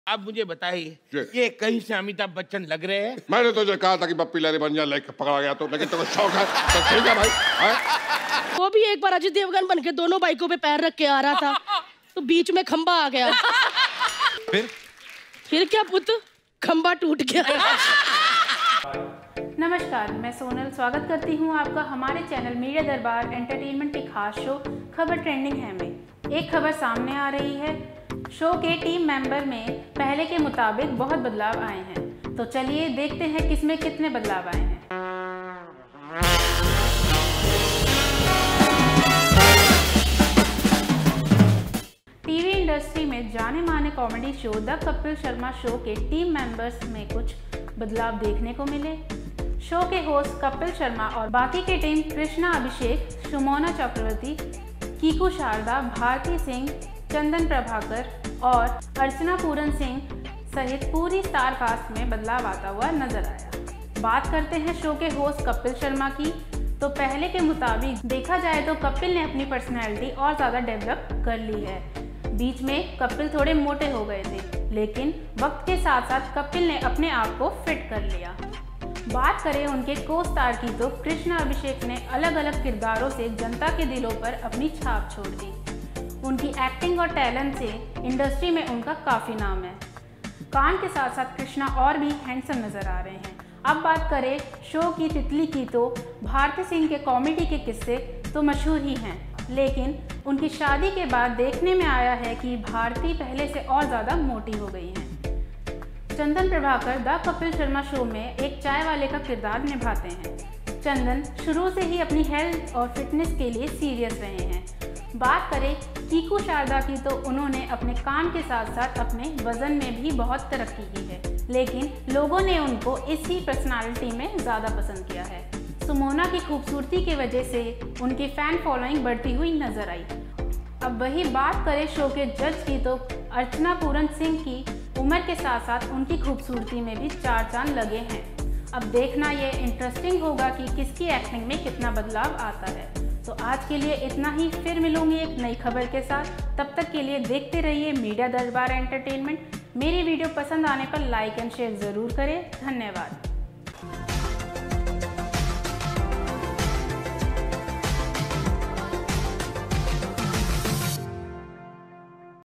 The Kapil Sharma Show | जानिए ‘द कपिल शर्मा शो’ के स्टार कैसे दिखते है अब | Media Darbar
टीवी इंडस्ट्री में जानामाना कॉमेडी शो ‘द कपिल शर्मा शो’ के टीम मेम्बेर्स में कुछ बदलाव देखने को मिले | शो के होस्ट कपिल शर्मा और बाकी की टीम कृष्णा ,अभिषेक , सुमोना चक्रबोर्टी, किकु शारदा, भारती सिंह, चंदन प्रभाकर, और अर्चना पूरन सिंह, सारे स्टारकास्ट मेम्बेर्स में बदलाव आता हुआ नज़र आया | यह रिपोर्ट सोनल द्वारा पेश की गयी है |
Some changes were seen in the team members of the well-known comedy show 'The Kapil Sharma Show' in the TV industry. The show's host Kapil Sharma and the rest of the team Krishna, Abhishek, Sumona Chakraborty, Kiku Sharda, Bharti Singh, Chandan Prabhakar, and Archana Puran Singh, all starcast members saw a change. This report is presented by Sonal.